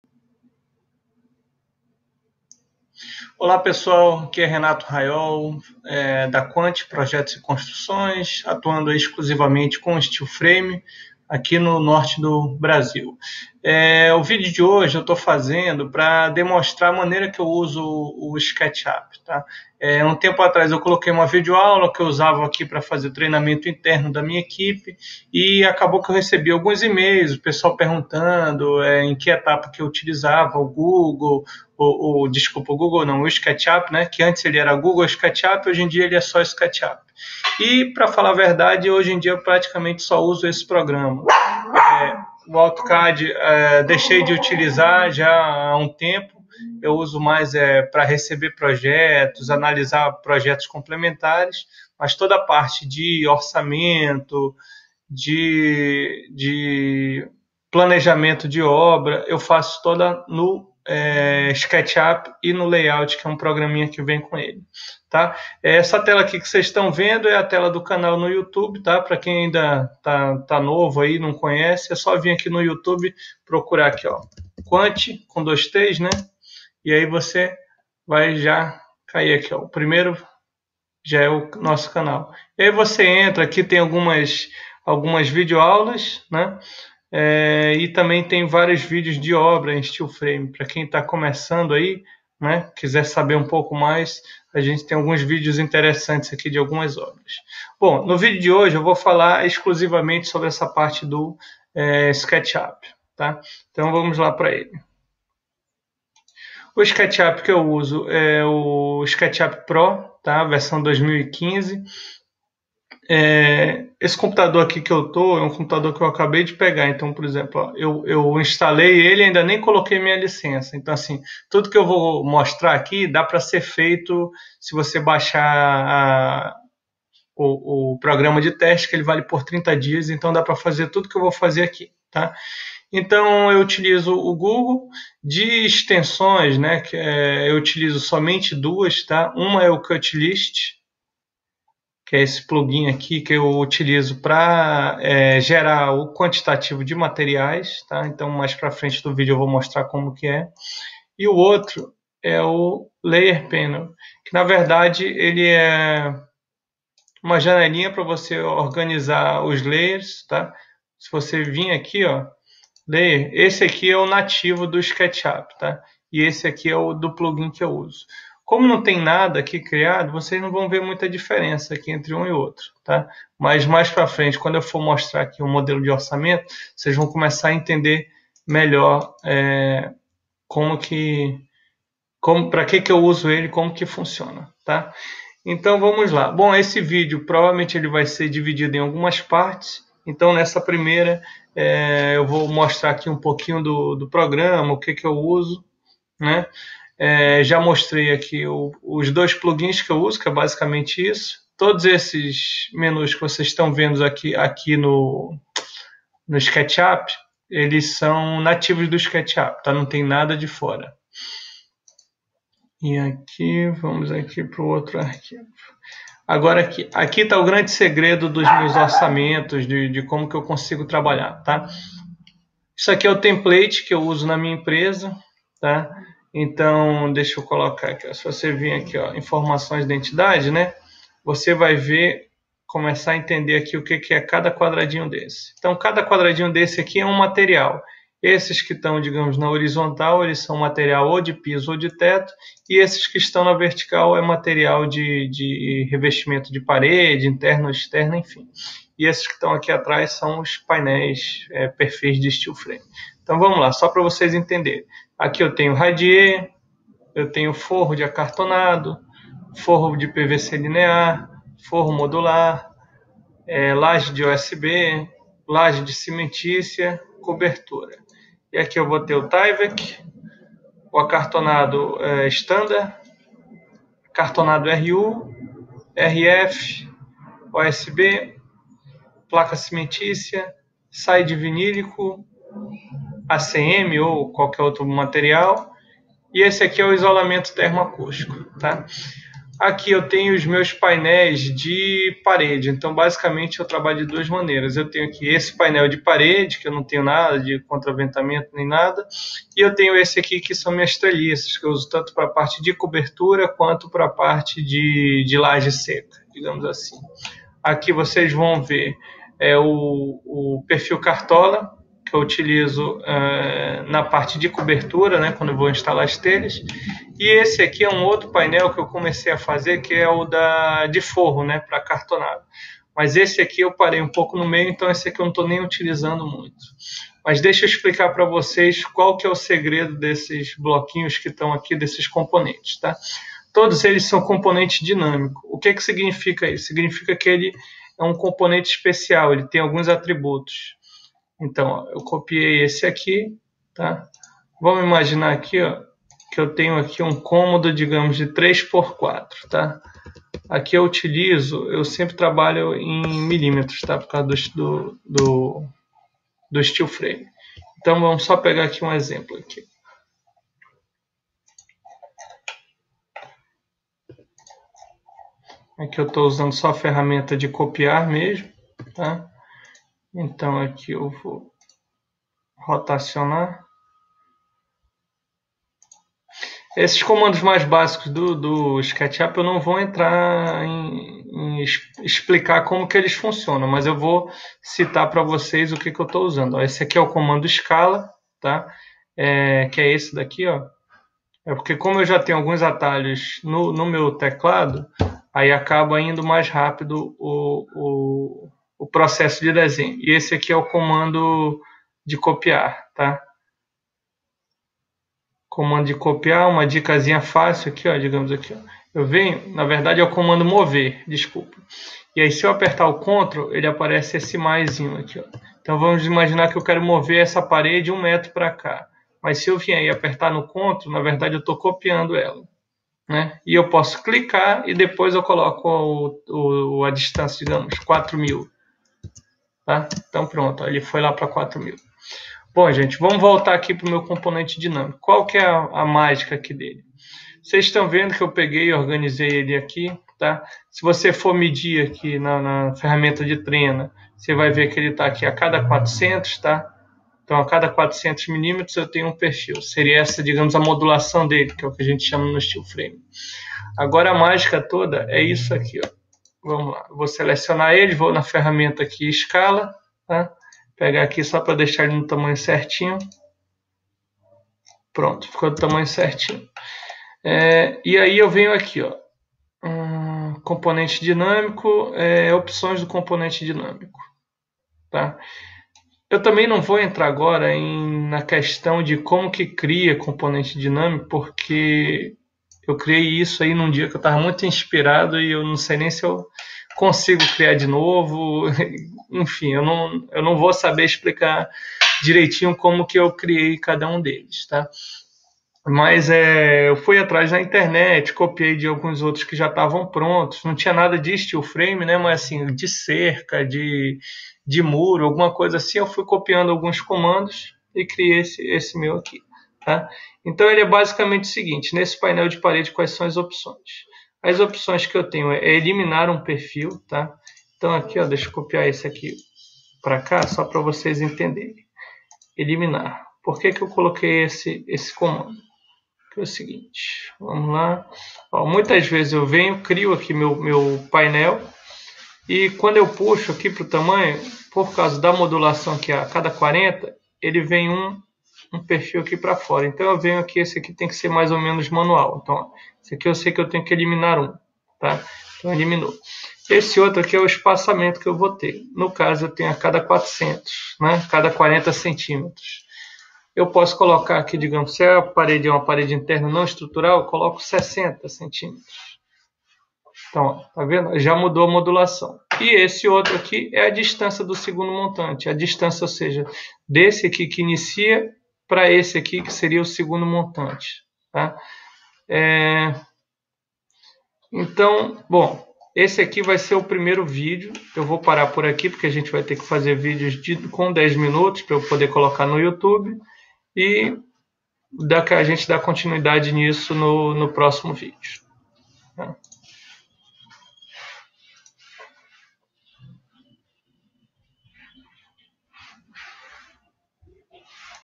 VIDEO AULA 01 - USANDO O SKETCHUP PARA MODELAR ESTRUTURAS LSF
Nesse vídeo você vai aprender as vantagens de se trabalhar com o software SketchUp para fazer levantamento de materiais e modelar a estrutura final de obras em Light Steel Frame.